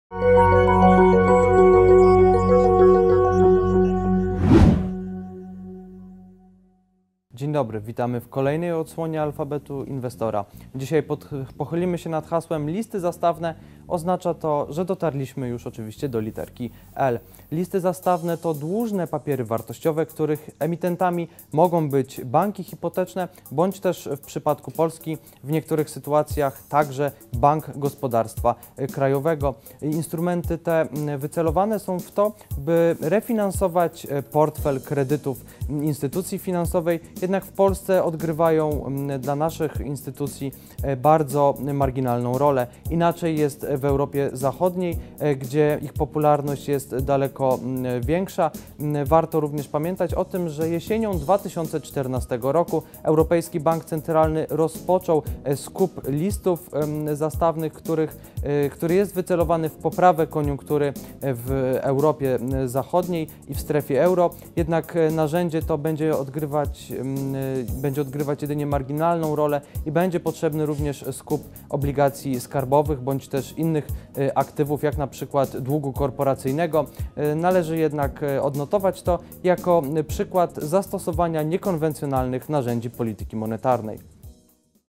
Alfabet Inwestora - litera L
W kolejnym odcinku Alfabetu zatrzymujemy się na literze "L" i omówimy hasło "listy zastawne". W odcinku spróbujemy przybliżyć to czym ten papier dłużny się charakteryzuje.